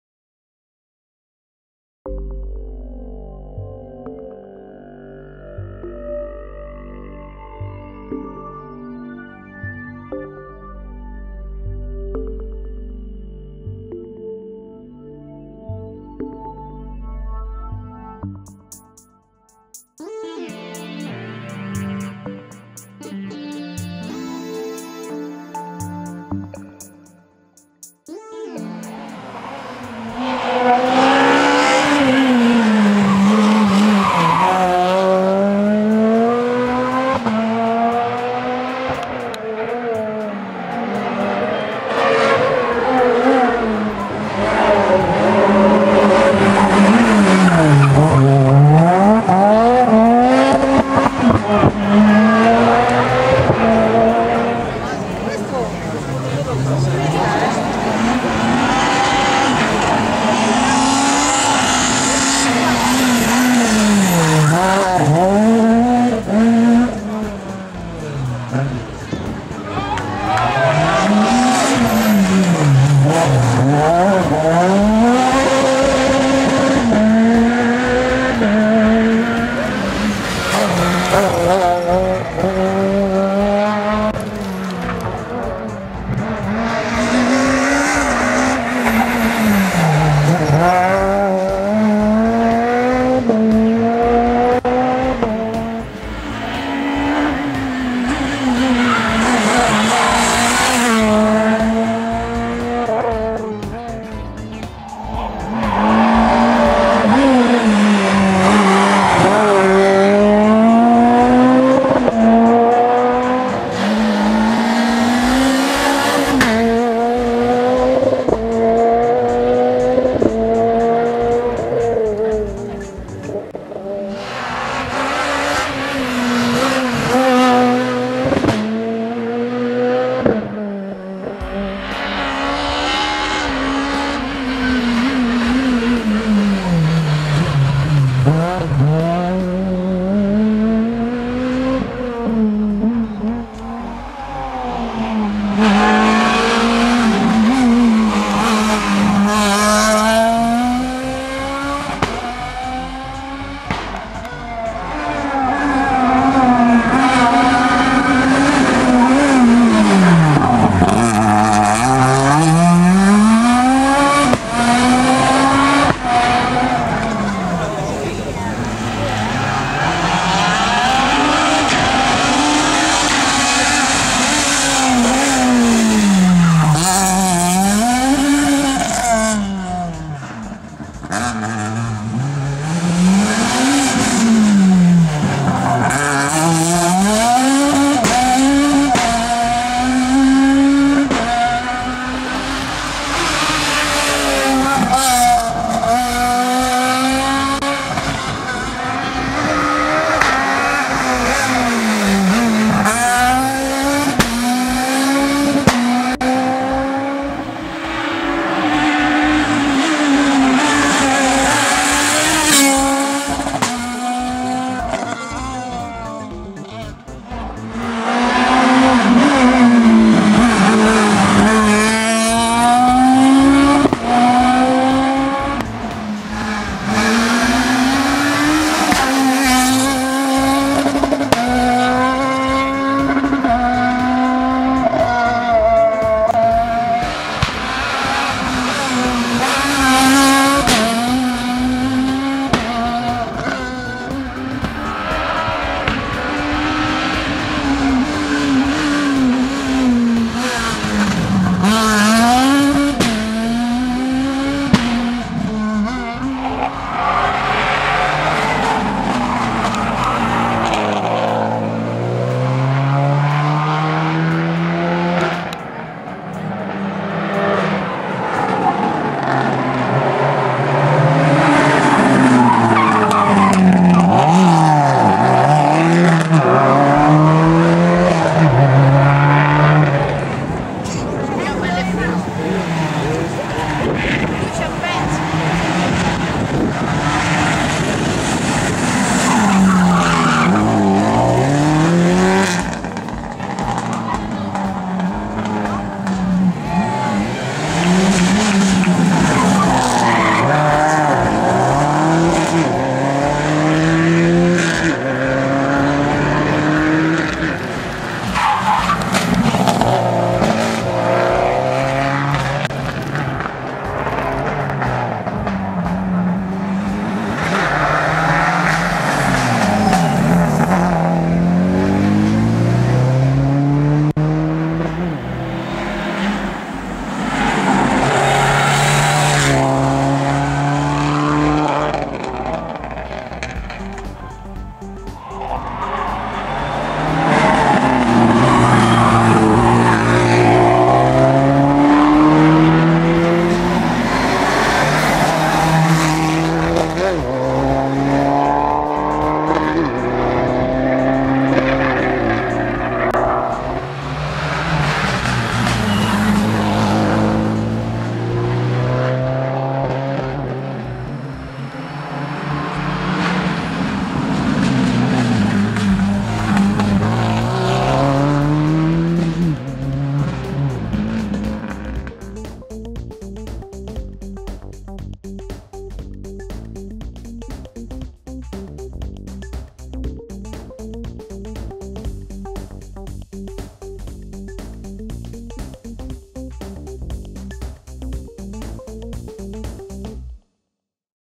Top Tre 1' RALLY DUE VIVAI PISTOIESI 2011
Le riprese dei primi tre classificati eseguite da Videorally.
E' disponibile il DVD con tutti passaggi degli equipaggi che hanno preso parte